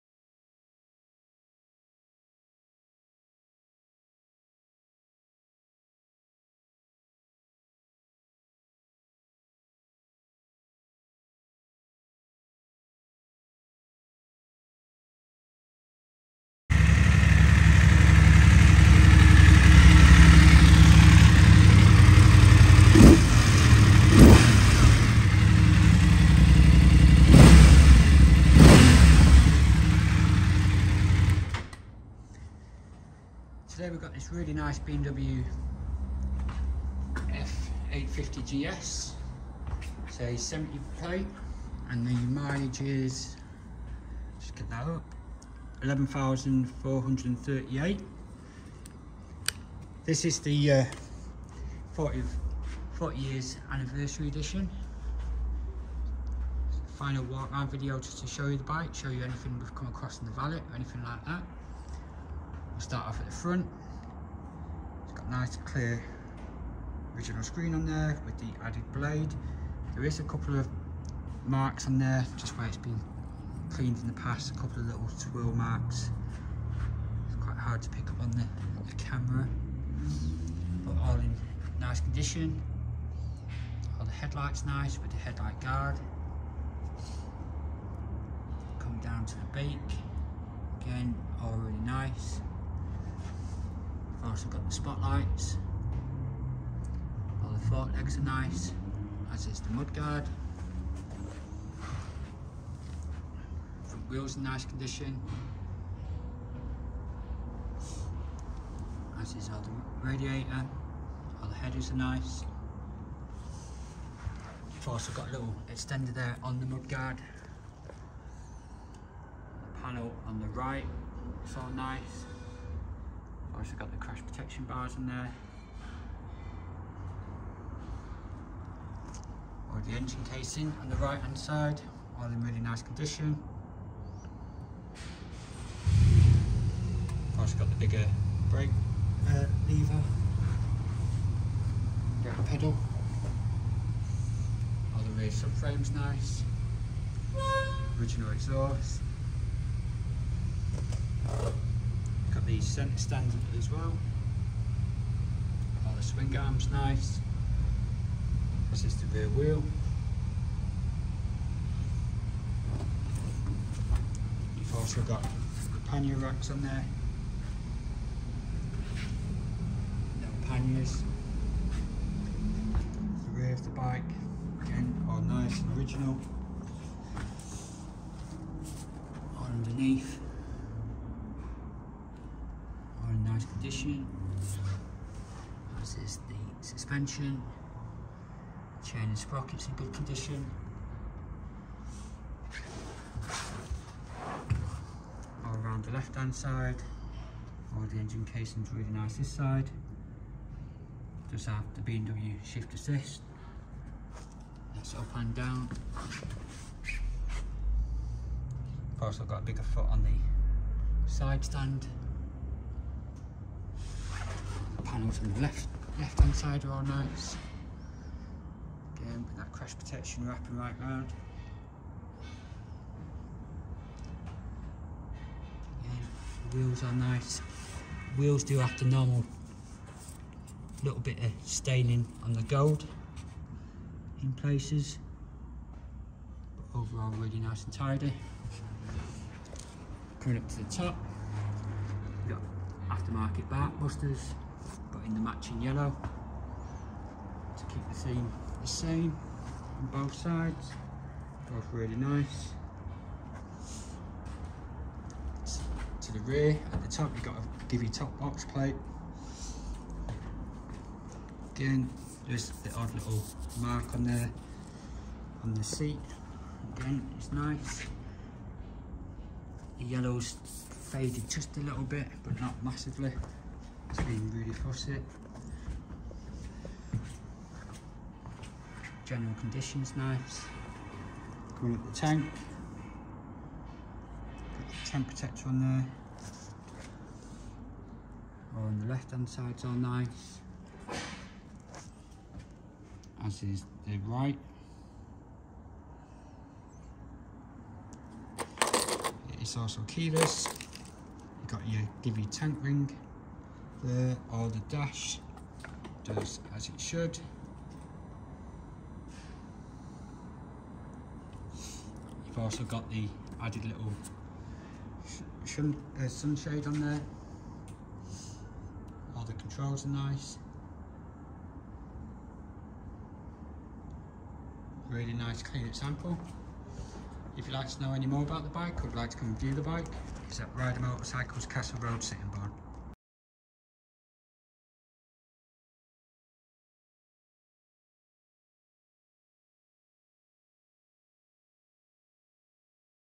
2020 BMW F850GS TE - Used Bike Video Tour
We have this 2020 BMW F850GS TE in stock now. Come and view it for yourself in store or buy